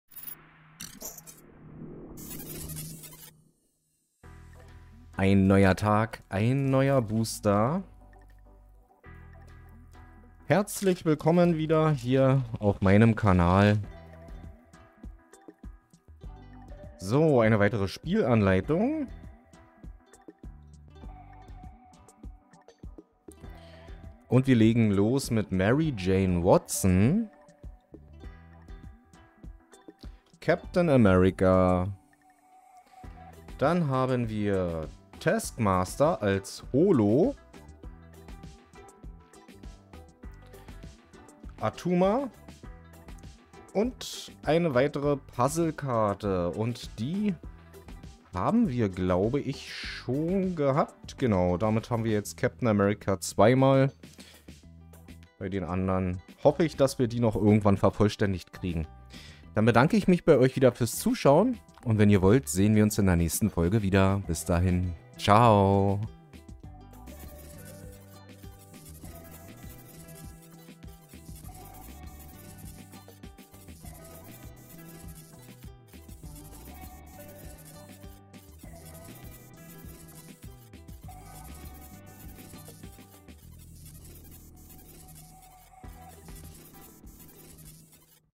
Der tägliche Booster 16 - Hero Attax (Topps) [324]
⬇⬇ VIDEOINFORMATIONEN ⬇⬇

Es ist Zeit für Helden!
Ich mag Filme in denen es um die Superhelden
von Marvel geht. Für mich ist dieses Universum
voller besonderer Helden und Bösewichte total
faszinierend.
Da stellt man sich doch manchmal die Frage:
"Was wäre wenn..?" 😏
Und wenn es dann besagte Figuren auch noch als
Sammelkarten gibt, bin ich Feuer und Flamme!
Lasst uns gemeinsam die Booster von Hero Attax
genießen. 🤩